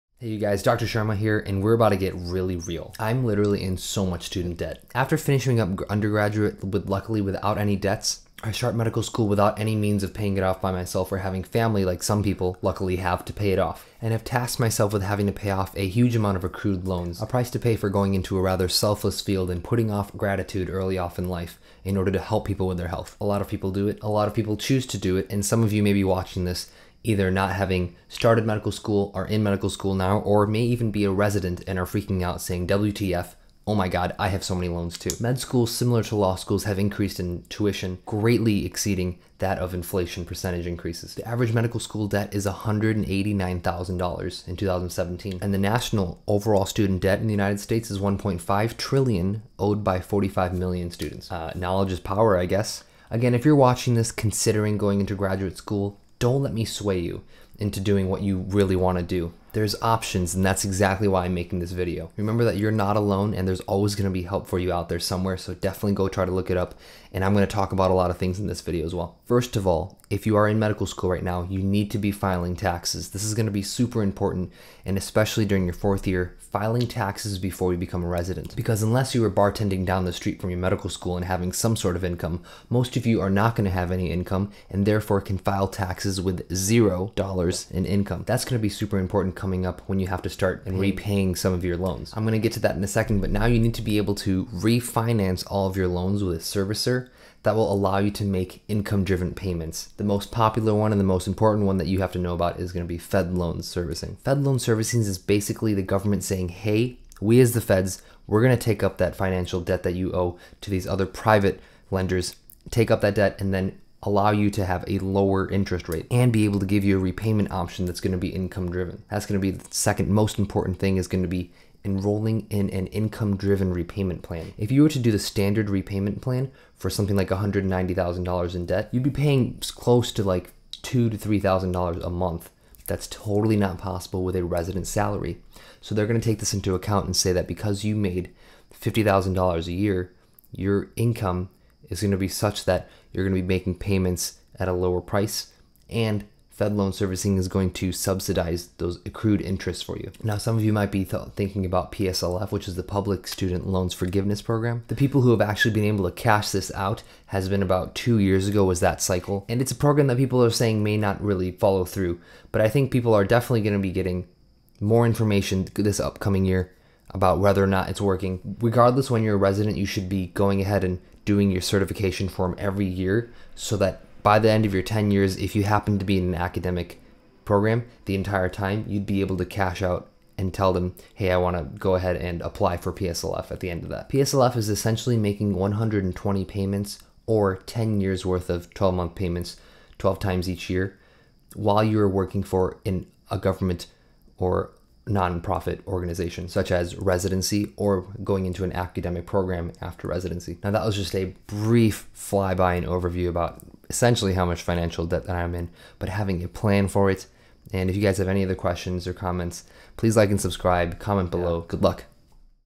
My Medical School Debt and Financial Plan !
Hey guys!
Student debt it is a REAL issue in America and the struggle is definitely real for those choosing to go into graduate school such as medical school !
Here's my discussion about my plans for my debt, such as income drive payments, PSLF, and just a general discussion about debt!

Hope this is helpful for you guys and good luck!